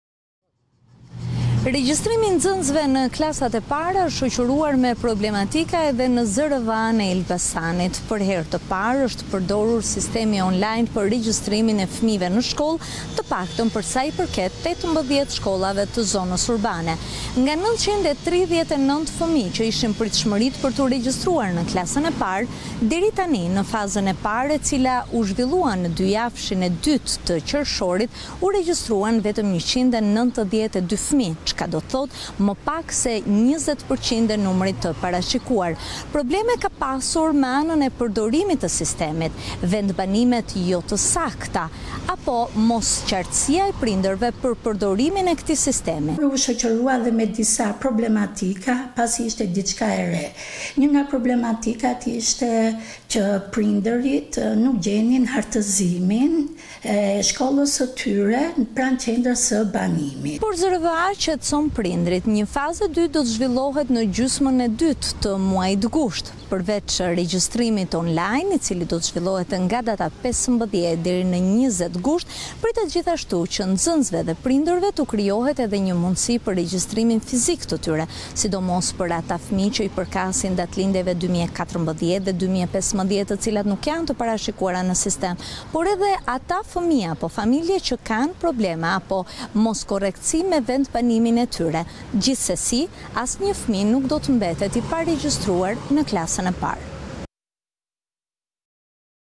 Top Channel/ Në Elbasan janë rregjistruar më pak se 20% e fëmijëve në klasën e parë
Regjistrimi i nxënësve në klasat e para është shoqëruar me problematika edhe në qarkun e Elbasanit.

Për herë të parë është përdorur sistemi ‘online’ për regjistrimin e fëmijëve në shkollë, të paktën për sa i përket 18 shkollave të zonës urbane.

Nga 939 fëmijë, që ishin pritshmëritë për tu regjistruar në klasën e parë, deri tani, në fazën e dytë që u zhvillua në 2-javëshin e dytë të qershorit, u regjistruan vetëm 192 fëmijë, ç’ka do të thotë, më pas se 20% e numrit të parashikuar. Probleme ka pasur me anën e përdorimit të sistemit, vendbanimet jo të sakta, apo mos qartësia e prindërve për përdorimin e këtij sistemi. Në disa shkolla gjithashtu nuk ka pasur kredenciale për hapjen e sistemit e-Albania.

Por ZRVA qetëson prindërit, një fazë e dytë do të zhvillohet në gjysmën e dytë të muajit Gusht. Përveç regjistrimit ‘online’, i cili do të zhvillohet nga data 15-20 gusht, pritet gjithashtu që nxënësve dhe prindërve t’u krijohet edhe një mundësi për regjistrimin fizik të tyre, sidomos për ata fëmijë që i përkasin datëlindjes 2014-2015 të cilat nuk janë të parashikuara në sistem, por edhe ata fëmijë apo familje që kanë probleme apo moskorrektësi me vendbanimin e tyre.

Gjithsesi asnjë fëmijë nuk do të mbetet i paregjistruar në klasën e parë.